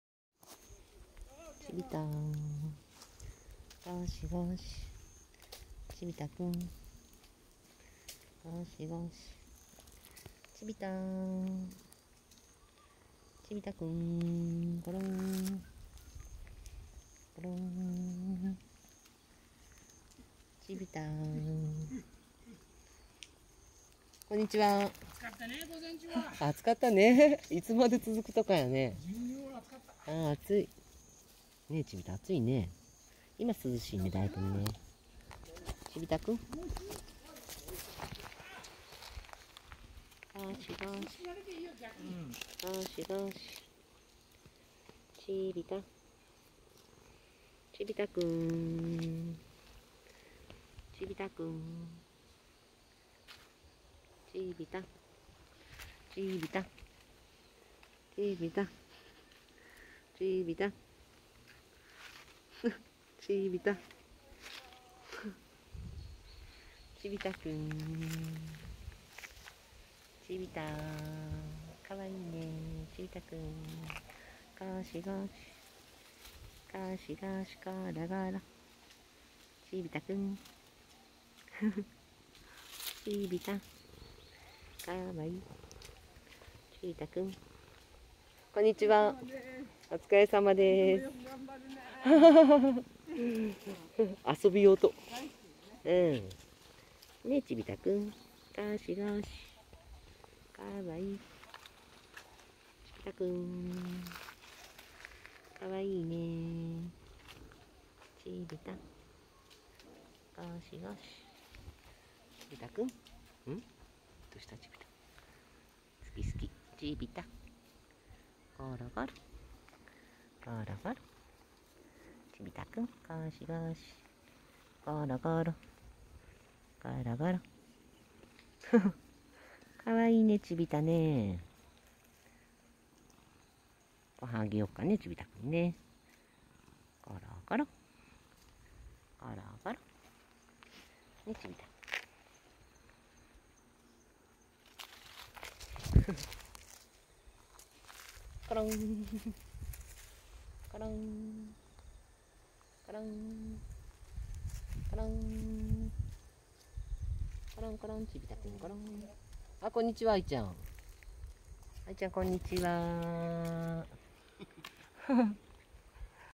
とにかく可愛いチビ太君！スリスリ、ゴシゴシ、ゴローン可愛いんだニャン❤
とにかく可愛いチビ太君！
スリスリ、ゴシゴシ、ゴローン！
可愛いんだニャン❤

Anyway, cute Chibita-kun!  suri suri goshi goshi goron ⤴️ It's cute Nyan ❤